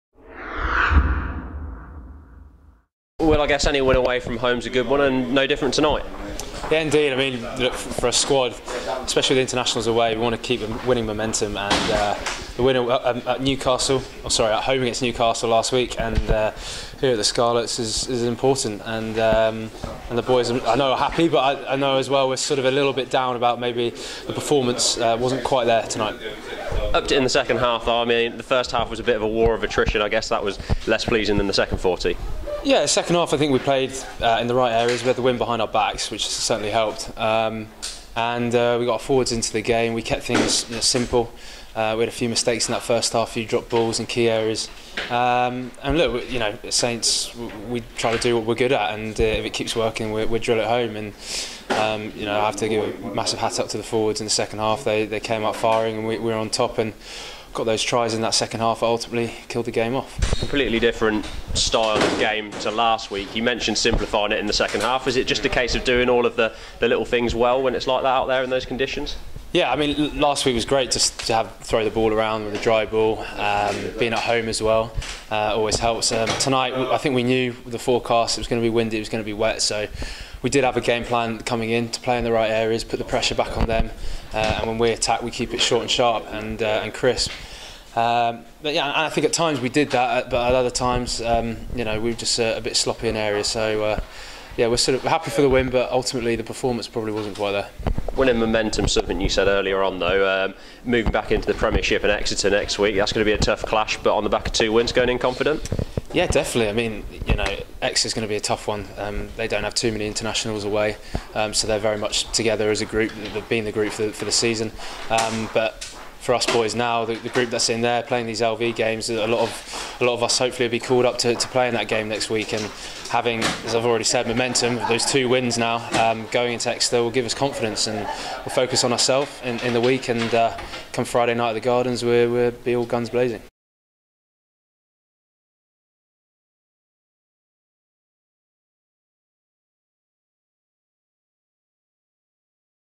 SCARLETS 7 SAINTS 24 Hooley: We need to step it up again for Exeter
While the Saints were delighted to leave West Wales with another win under their belts, LV= man of the match Will Hooley was honest in his assessment of their performance, during the first 40 in particular.

The fly half called for a step up in performance next Friday as first welcome third in the Aviva Premiership to Franklin's Gardens.